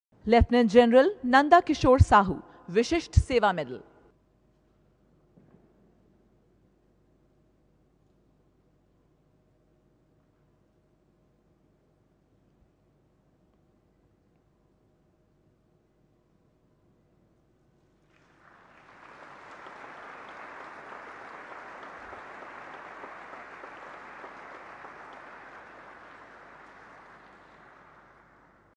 President Kovind presents Param Vishisht Seva Medal to Lieutenant General Nanda Kishore Sahoo
President Kovind presents Param Vishisht Seva Medal to Lieutenant General Nanda Kishore Sahoo, VSM, The Army Dental Corps.